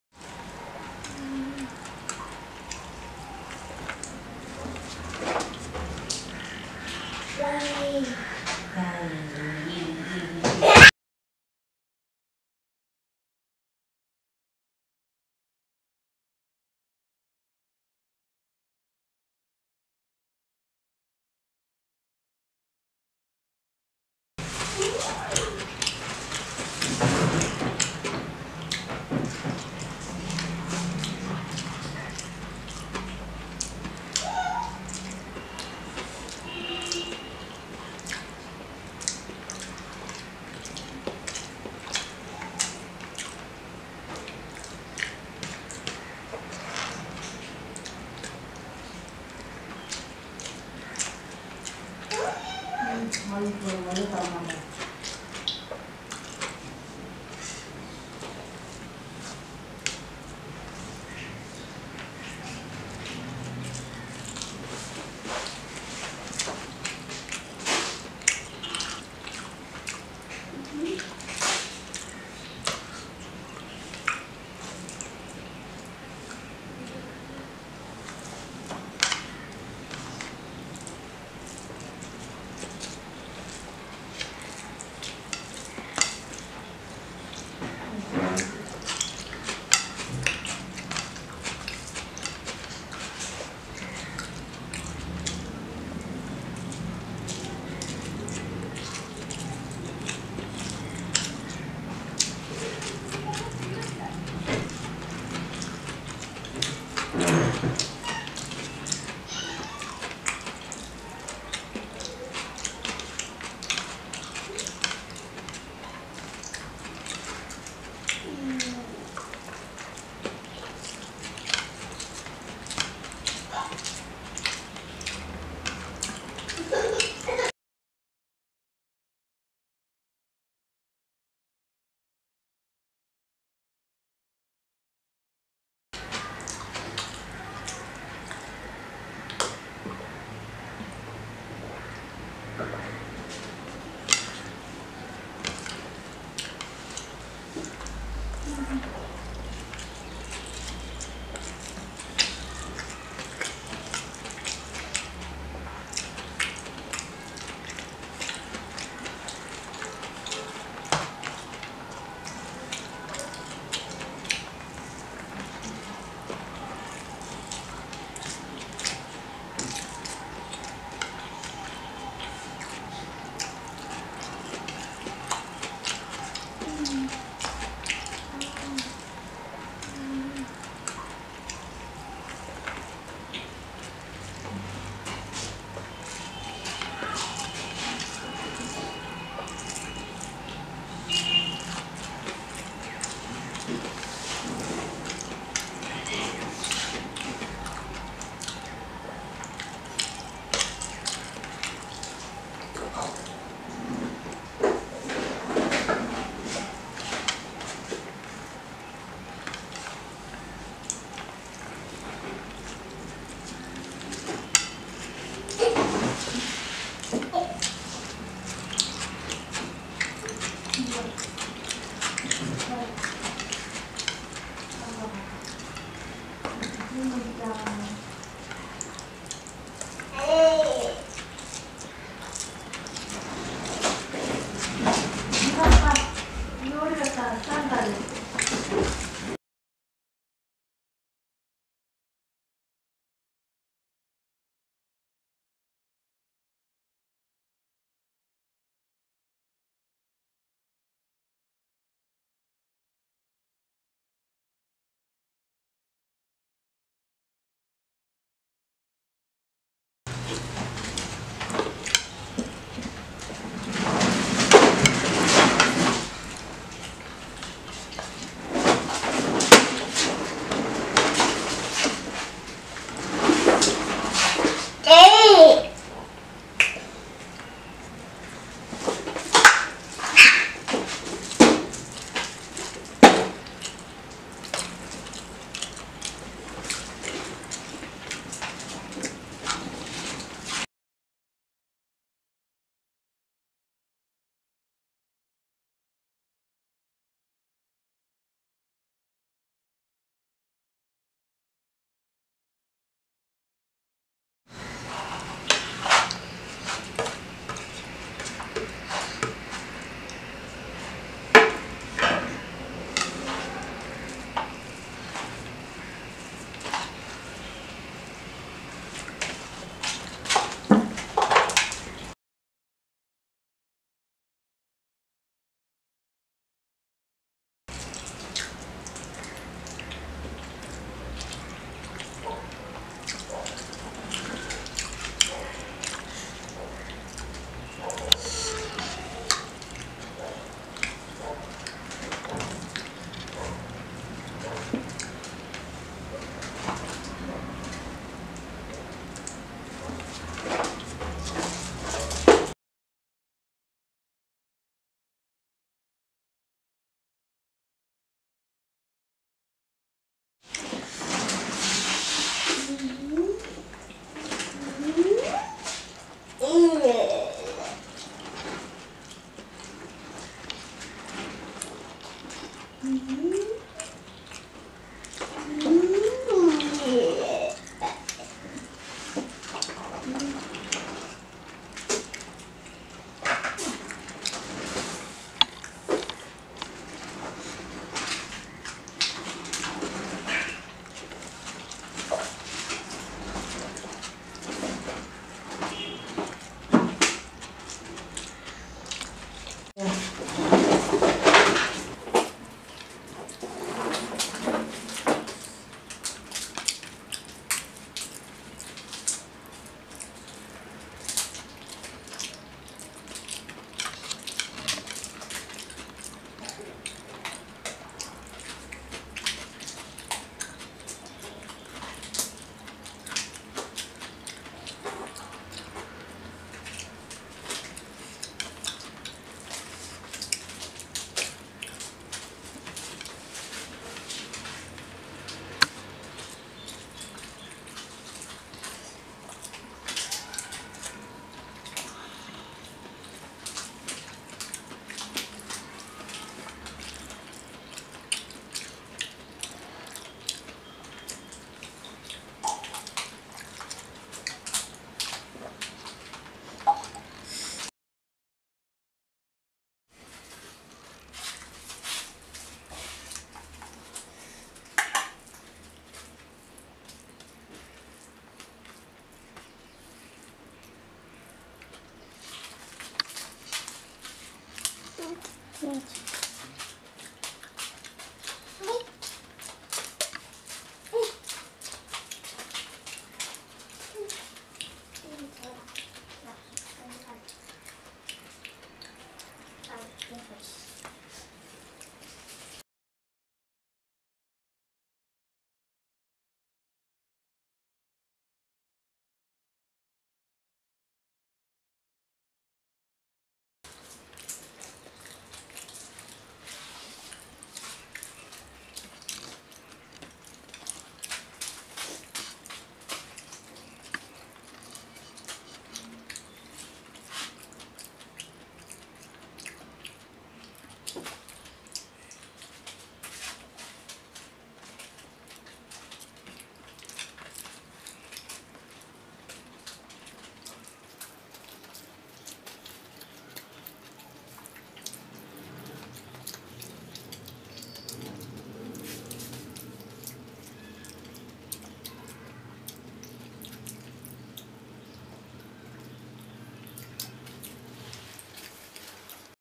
ASMR|Mushroom biryani|Califlower chilly|Eggs|raita eating challenge|Eating show|Biryani mukbang
hi friends
epudi irukenga
happy Aah irunga
video paarunga
enjoy panunga
support panunga